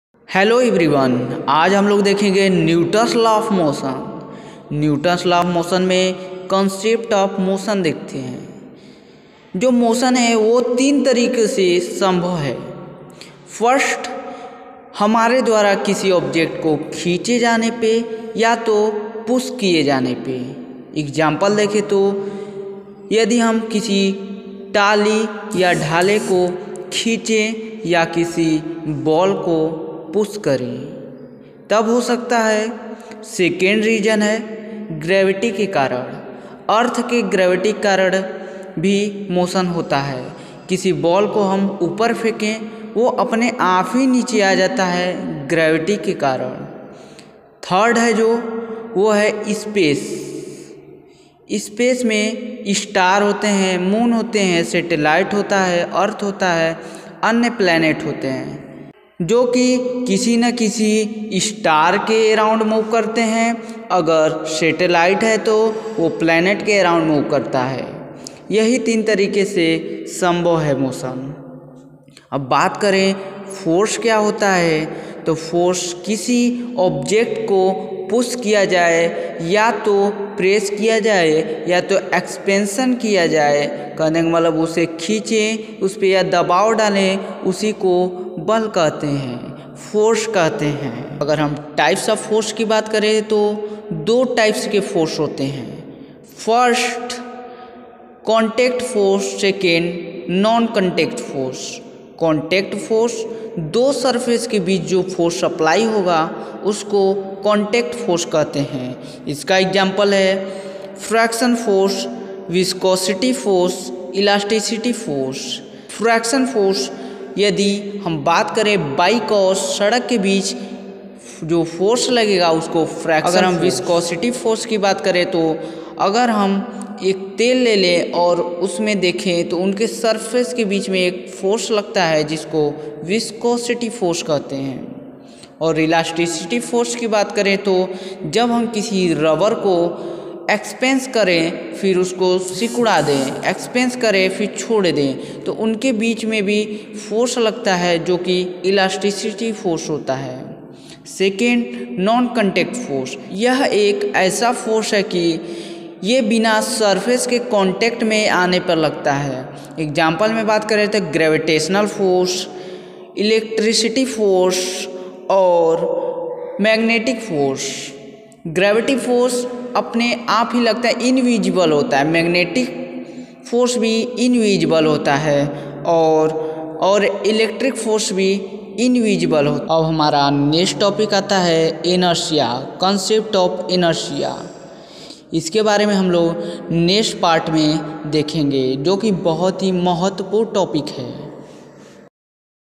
Newton's law of motion. Newton's first law. Newton's law of motion part 1
hello students hamane es video me Newton's law of motion ka frist part banaya hai
es video me hamane types of motion ke bare me bataya hai
and what is the force types of for ke bare me bhi bataya hai force ke 2 types hote hai first types contact force and 2 non contact force...